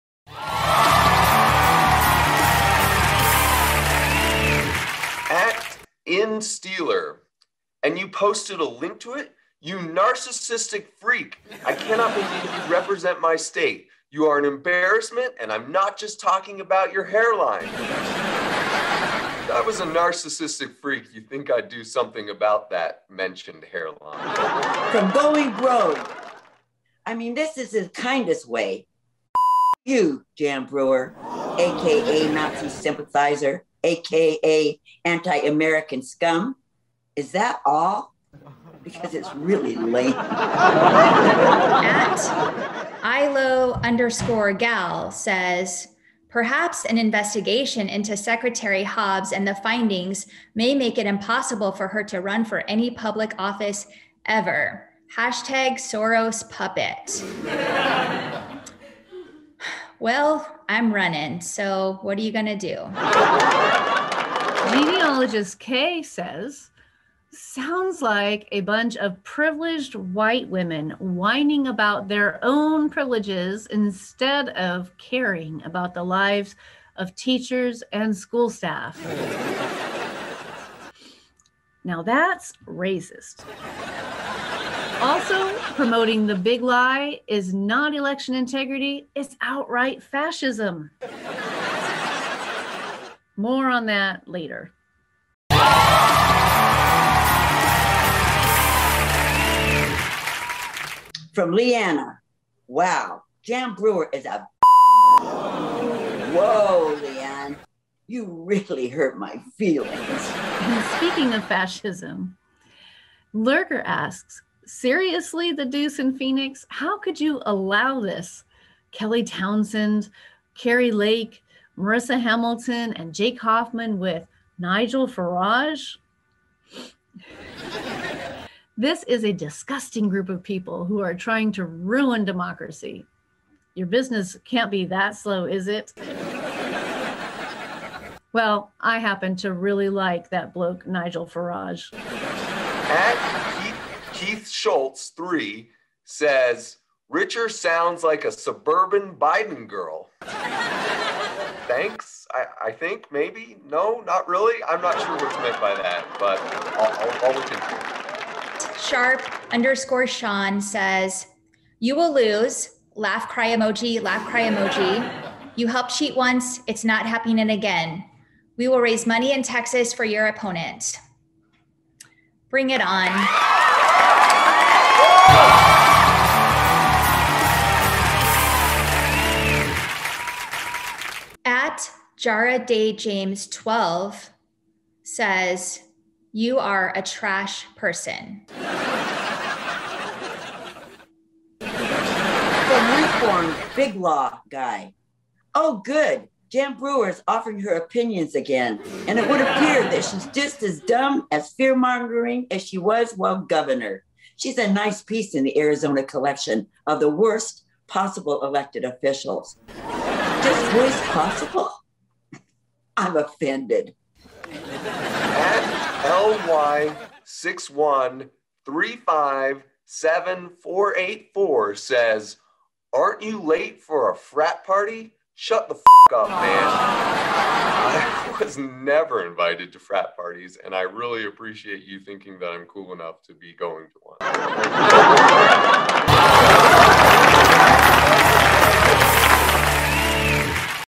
Mean Tweets - AZ Government Edition 2021
An raucous, un-censored (censored) look at some of the meanest tweets this side of the desert has ever (this year) seen!